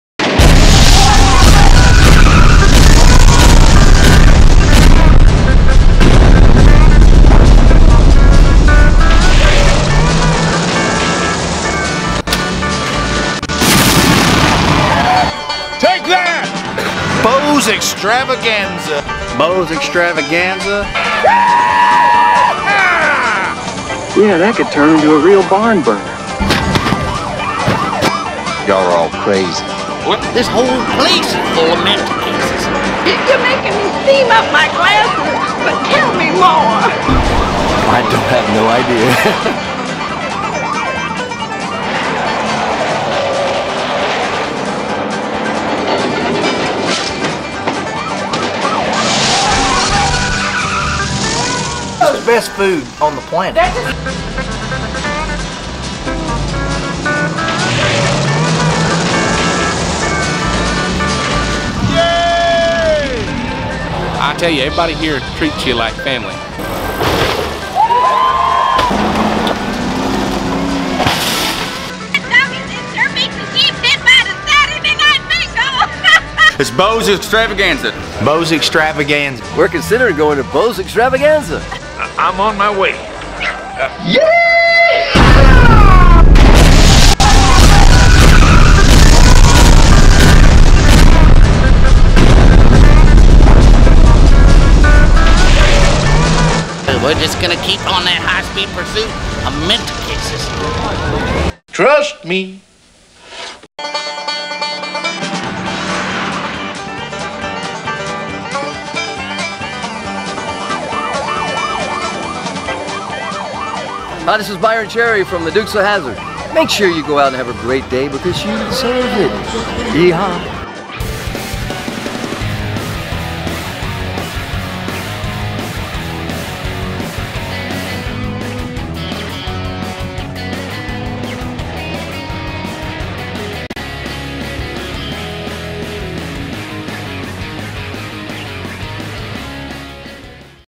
Bo's Extravaganza 2021 COME SEE US!!
Bo's Extravaganza 2021 COME SEE US!! Bo's Extravaganza is April 10th and 11th, 2021 at John Schneider Studios in Holden, Louisiana. Come out and see me along with Mike Cook from Hollywood Prop Badges, John Schneider "Bo Duke" of The Dukes of Hazzard, Byron Cherry "Coy Duke" of The Dukes of Hazzard and many others. Concerts by Keith Burns of Trick Pony and Cody McCarver of Confederate Railroad just to name a few. There will be stunts, a carnival, car show and more stunts. John will be filming scenes for his movie "Poker Run" at the event. Come be in a movie. Get your tickets now!